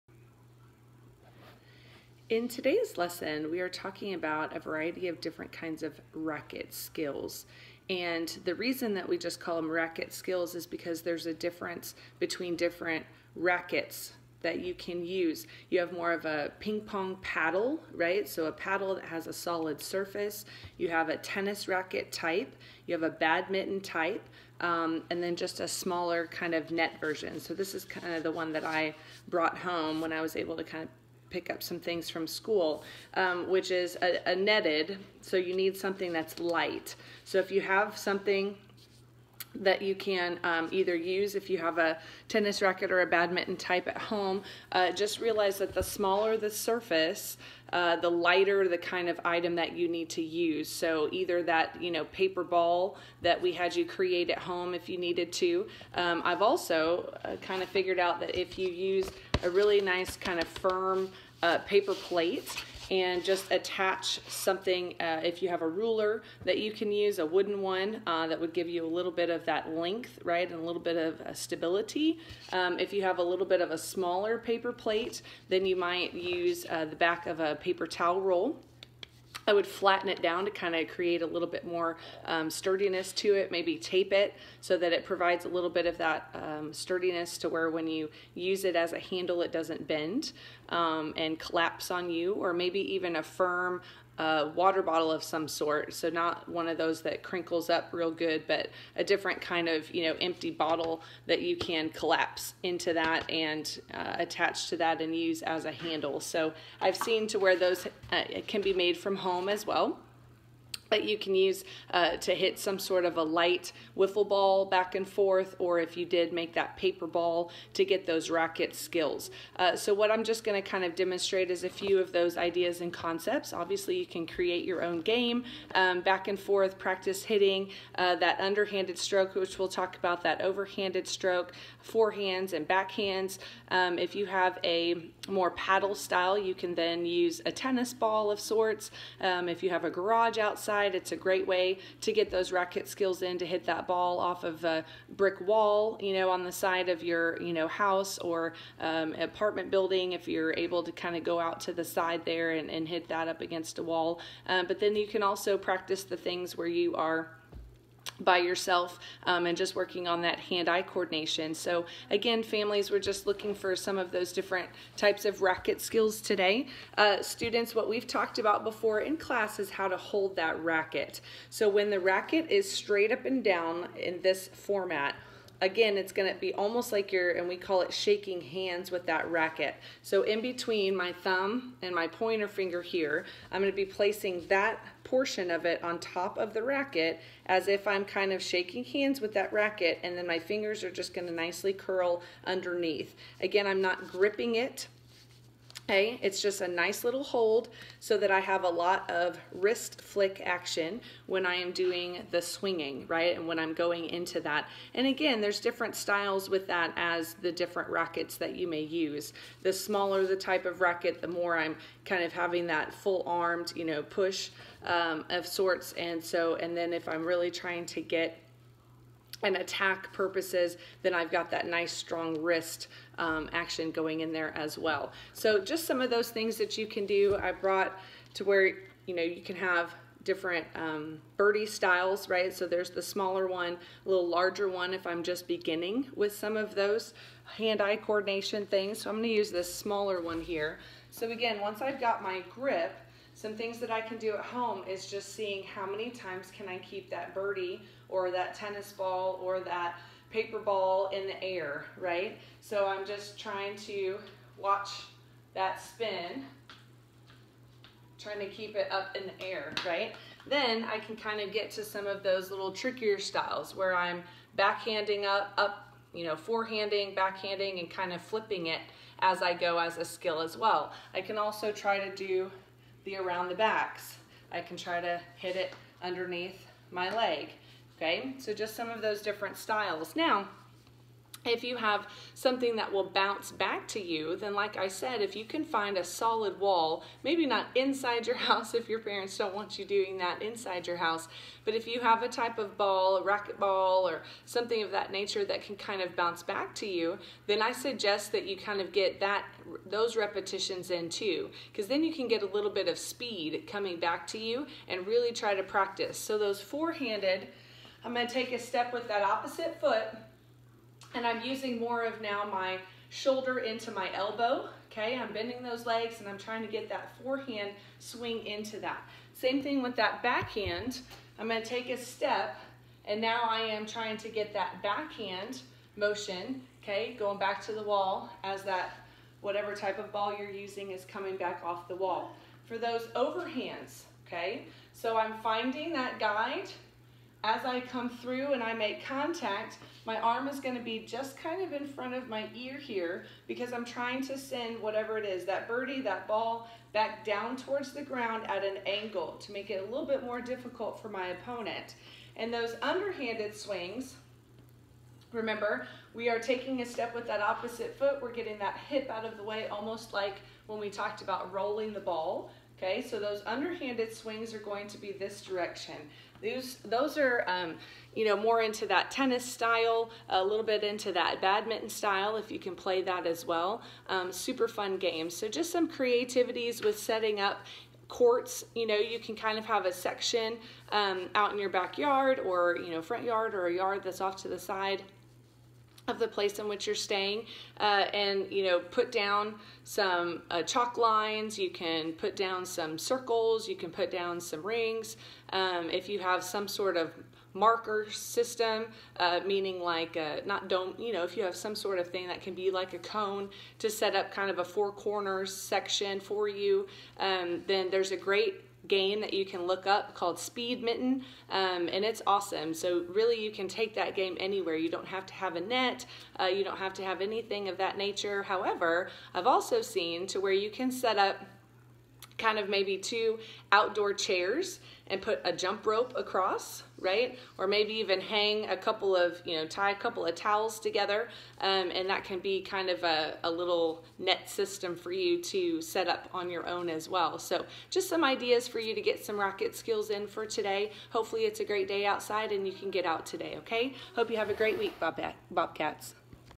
Racquet Skills
This video goes over the basics of racquet movements, gives a variety of uses for different racquets and examples of activities/racquets to use from home.